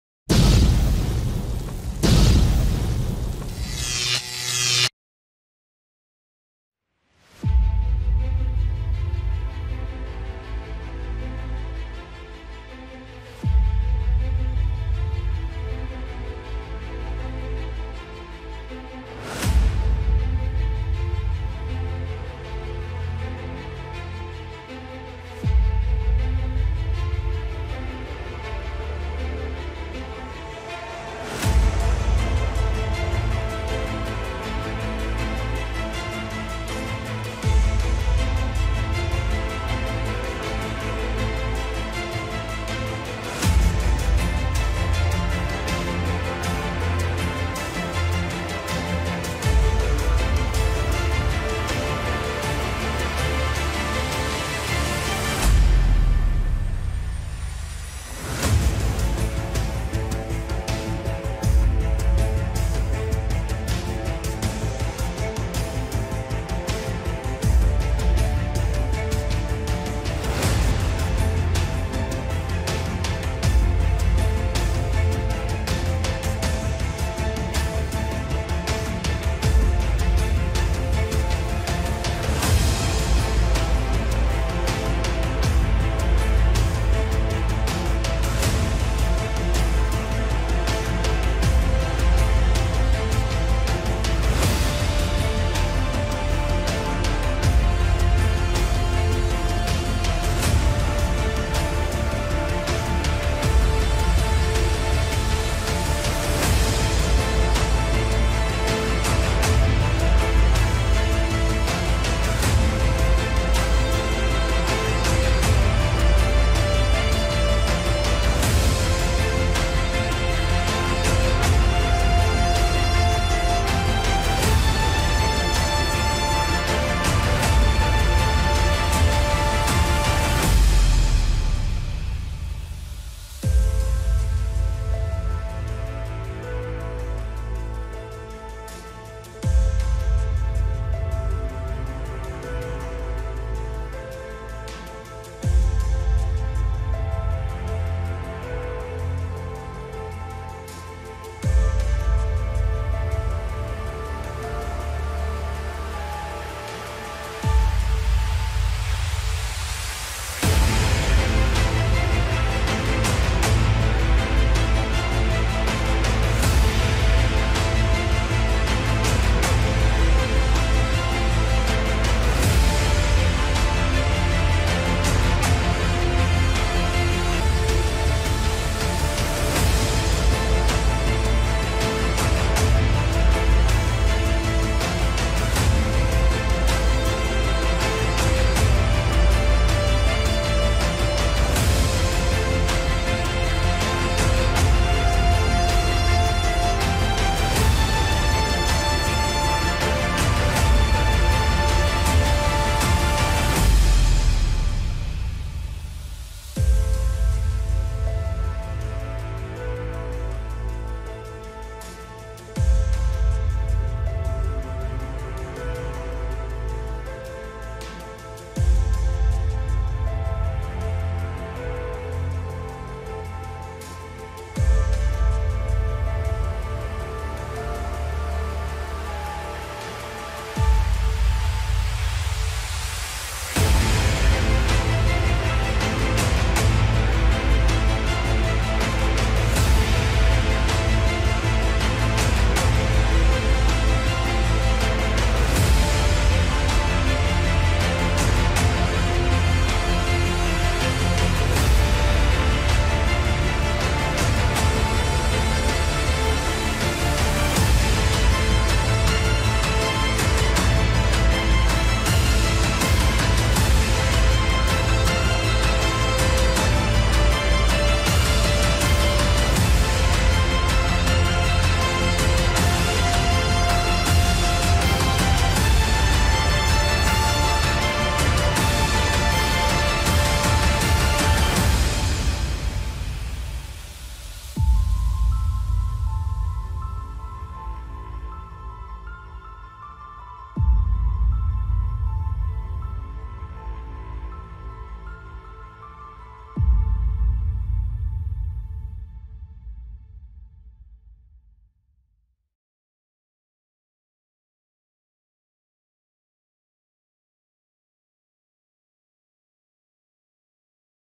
USA vs Italy Military Power Comparison - 2022
In this video we did a military comparison between two countries to see who is more powerful.
USA and Italy.

In this video, we compared the land, air and naval forces of both countries.
Images in the video are representative.